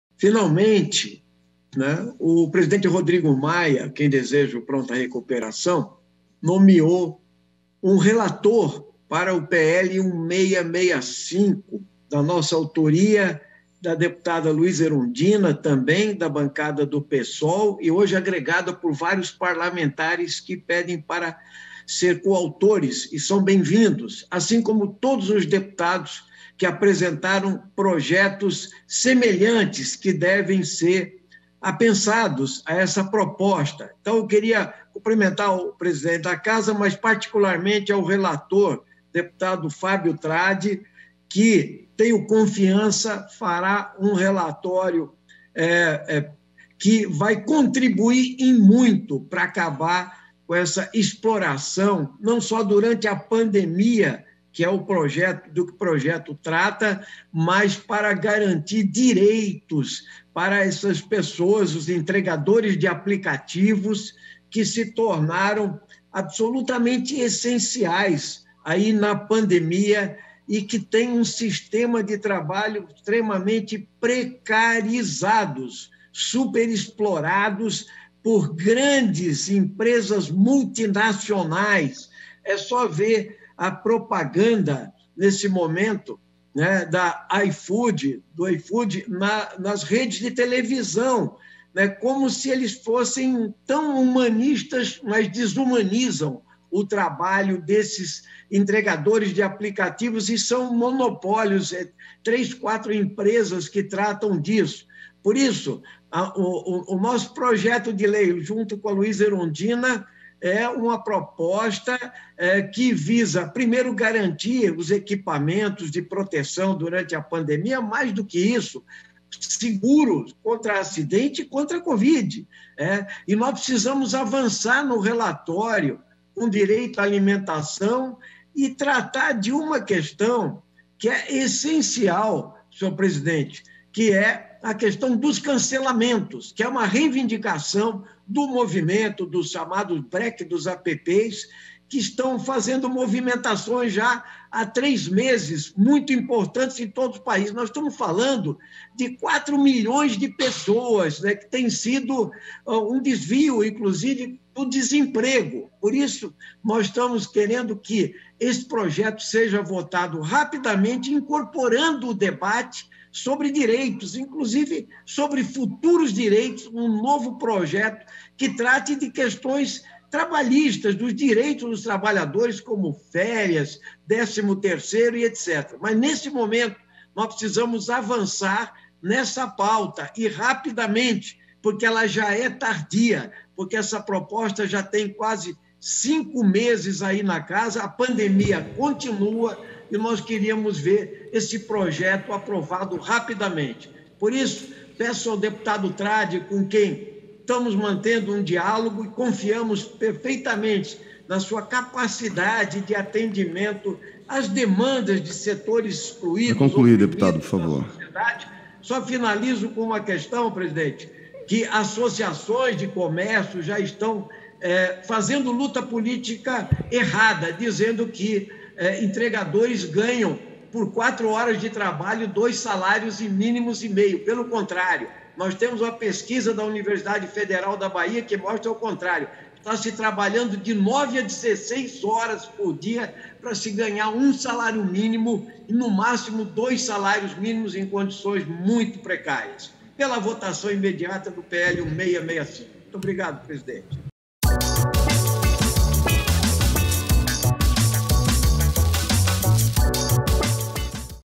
21/09/20 | Pela votação imediata do PL 1665/20 - PL dos Entregadores
Já foi nomeado relator do PL de nossa autoria em conjunto com a companheira Luiza Erundina. Rodrigo Maia entregou para Fábio Trad (PSD-MS) essa tarefa.

Agora é hora de pressionar para que venha o quanto antes para apreciação do Plenário e eventual aprovação. Os trabalhadores e trabalhadoras dos aplicativos de entrega não podem mais esperar.